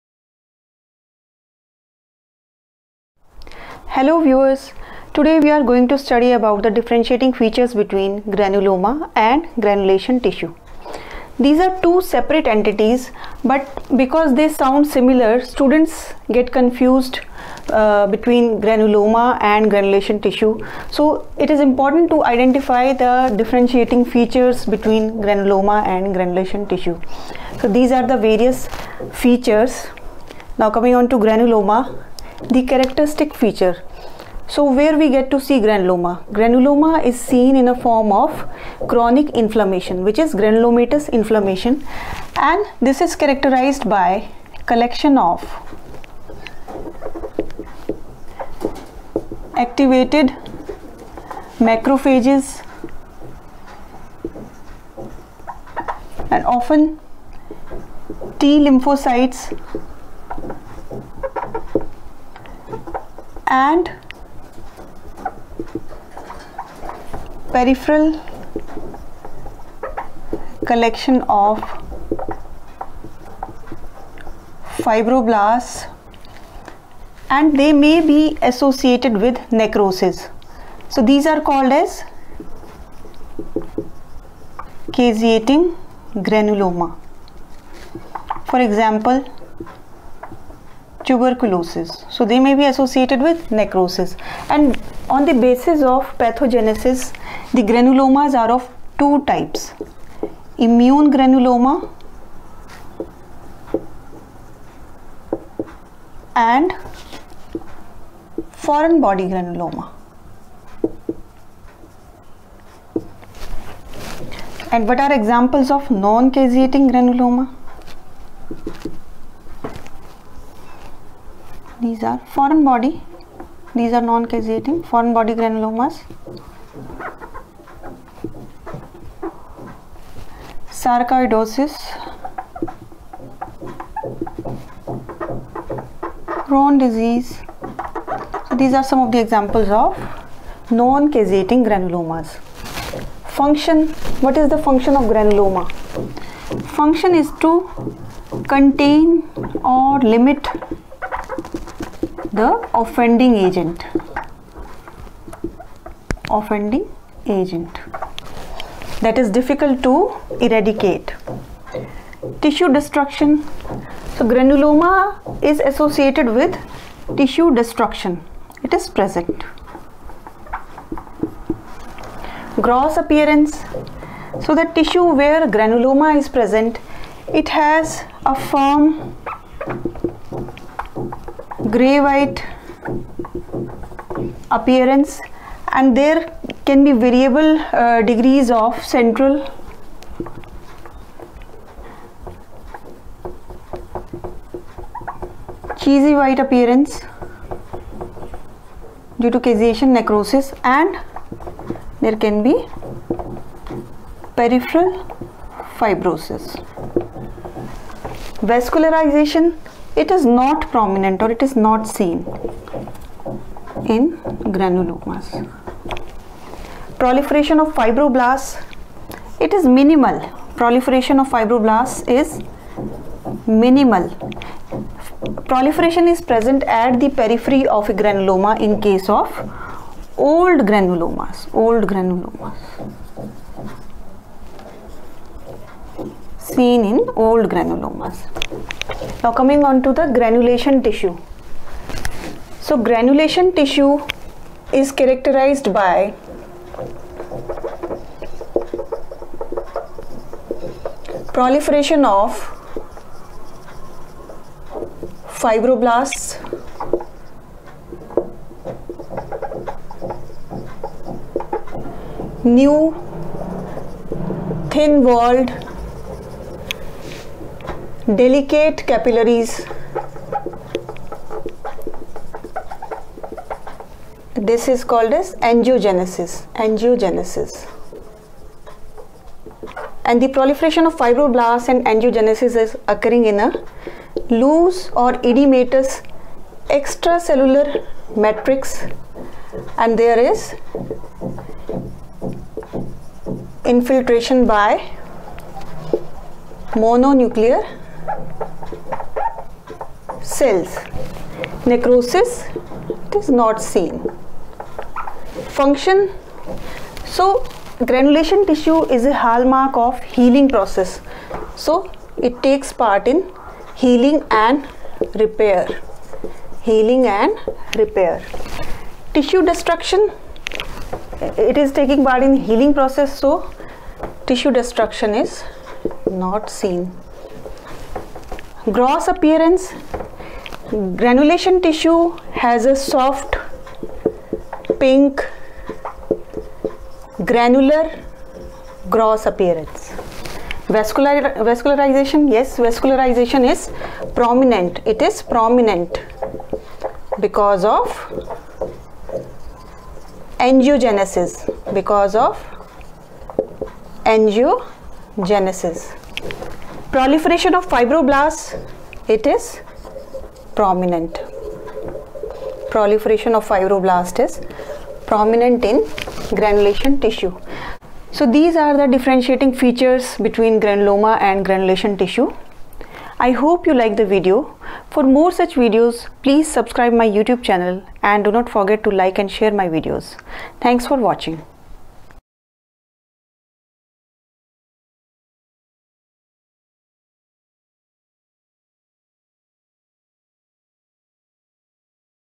Granuloma vs Granulation tissue: Differentiating features between Granuloma and Granulation tissue
This video lecture; Granuloma vs Granulation tissue describes the differentiating features between Granuloma and Granulation tissue in a tabulated form.

Dr. Shweta Rana Khokhar